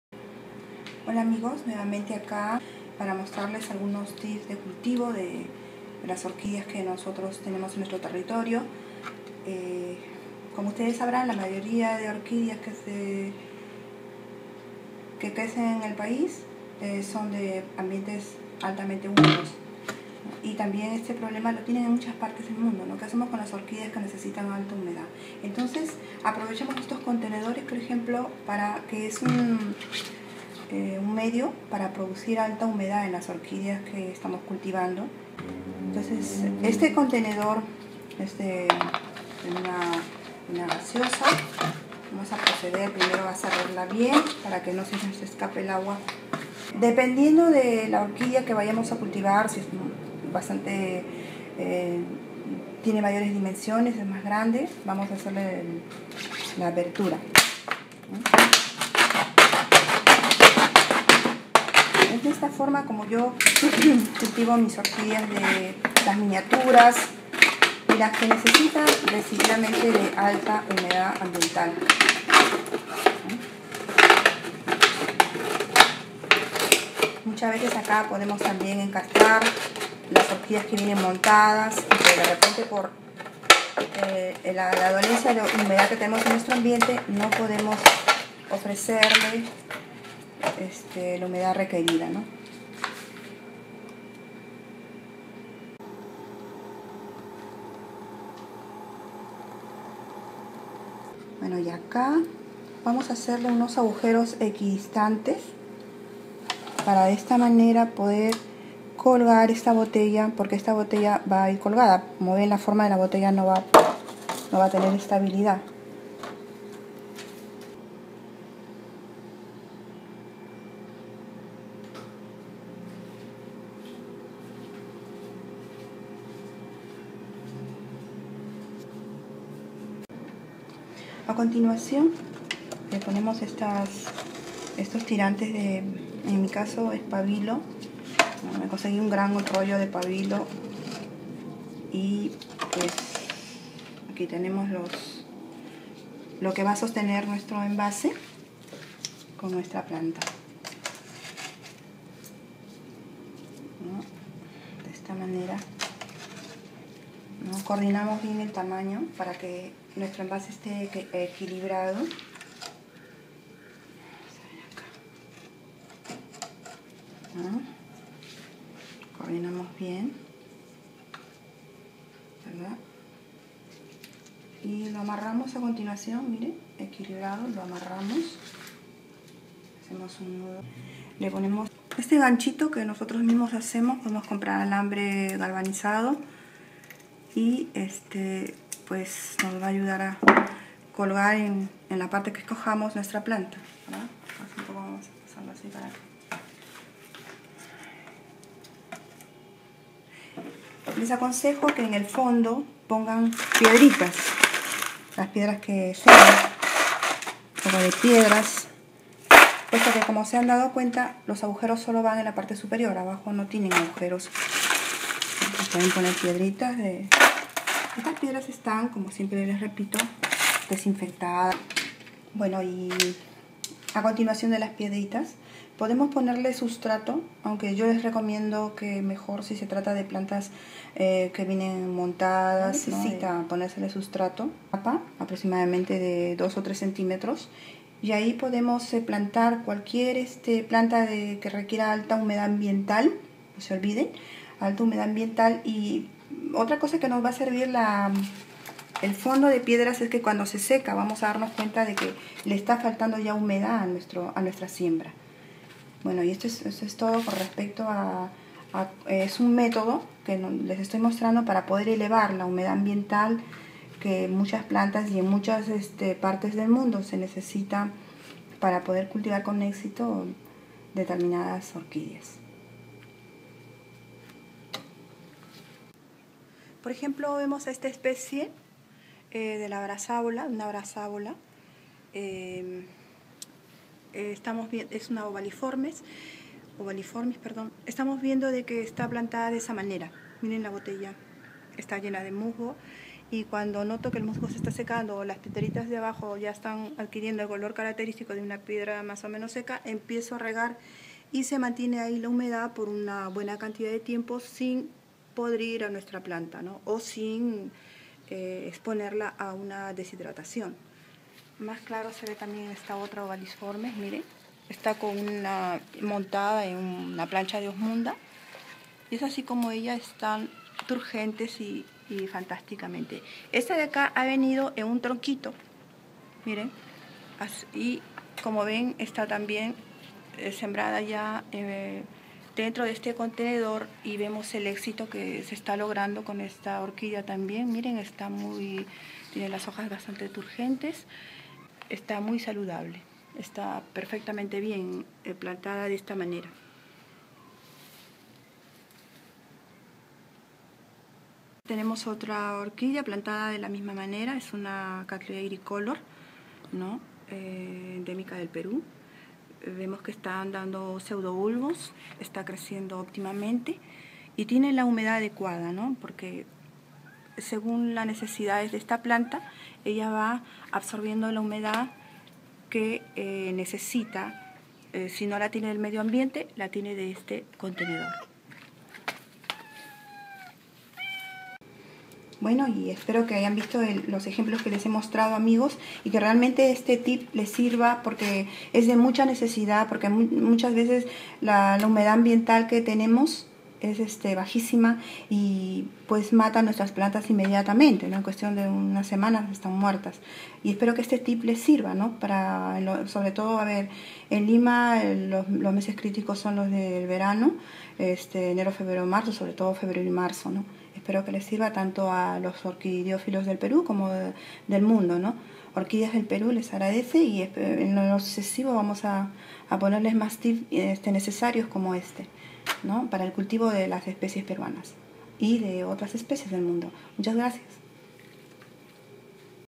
Como aumentar la humedad ambiental en nuestras orquideas
Las Phalaenopsis muestran un hábito de desarrollo monopodial. No tienen pseudobulbos. Las raíces son gruesas y están recubiertas por un tejido esponjoso llamado velamen que ayuda a la absorción de agua y nutrientes. Por dentro está la auténtica raíz, que contiene clorofila y presenta color verde. El rizoma se desarrolla erecto y en su extremo produce dos gruesas y carnosas hojas alternas y elípticas cada año. Las hojas basales más viejas se caen al mismo tiempo. La planta retiene de éste modo de cuatro a cinco hojas. Las hojas son de color verde oscuro, moteadas o con manchas azul grisáceo.